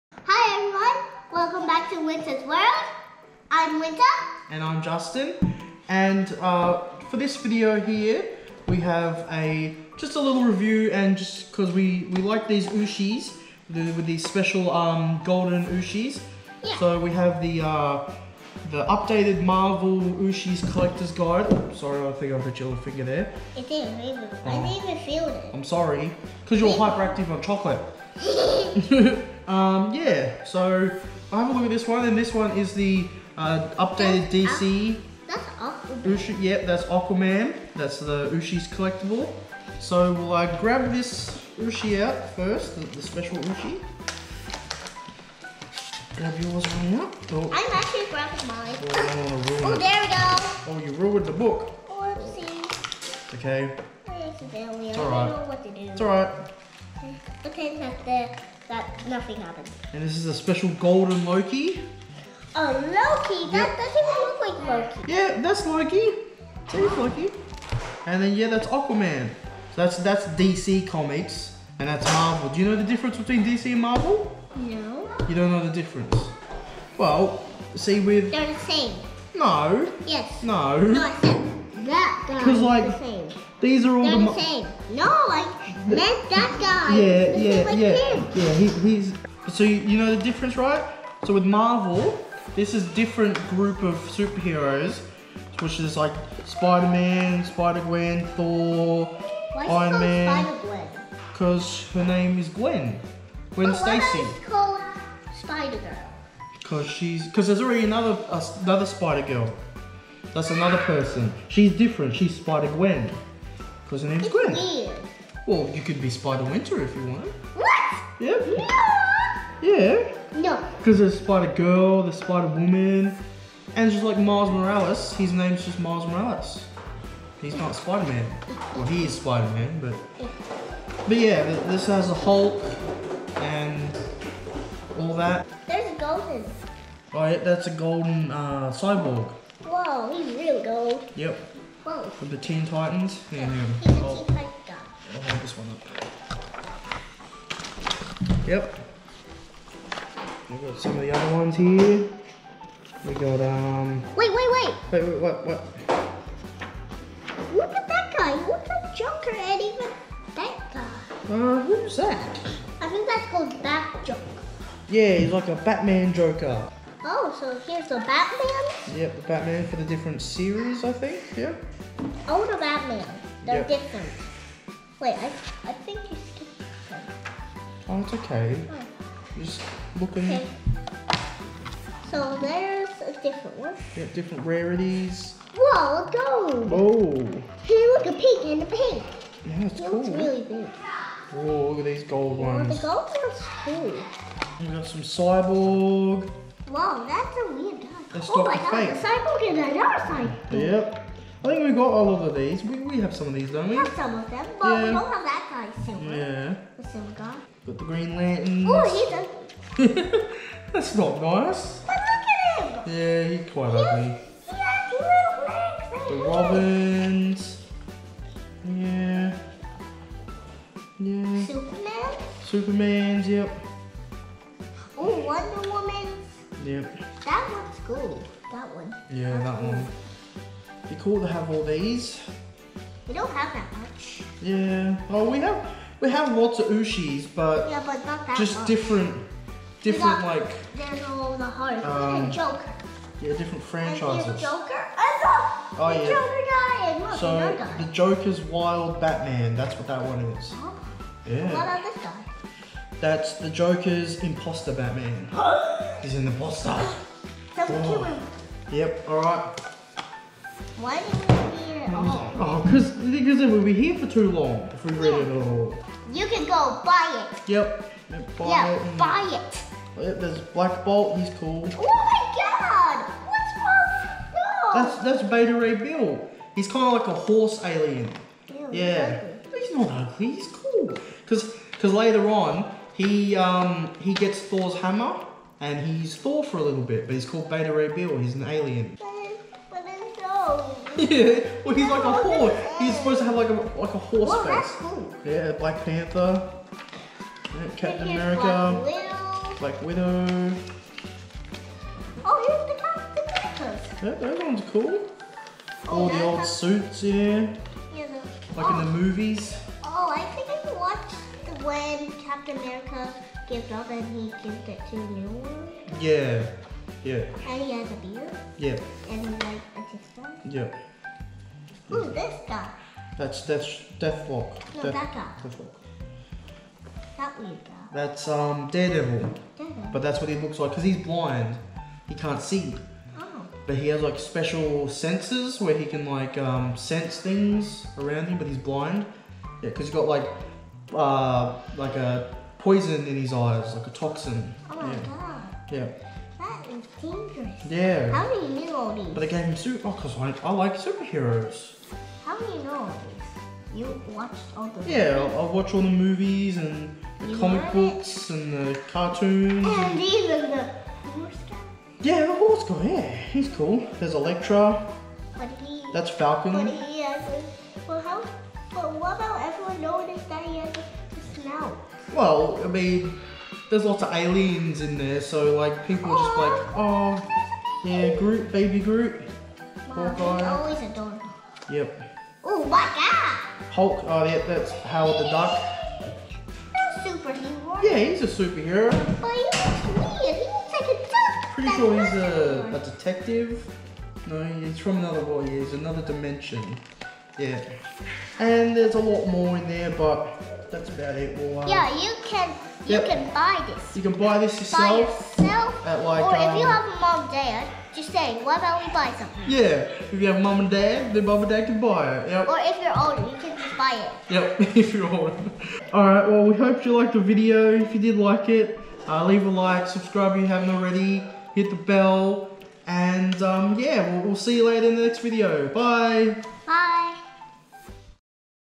Ooshies Marvel & DC Superheroes UPDATED Collectors Guide Opening And Review
In this video we open up both the Ooshies Marvel & DC Superheroes Updated Collectors Guide. We hope you enjoy :)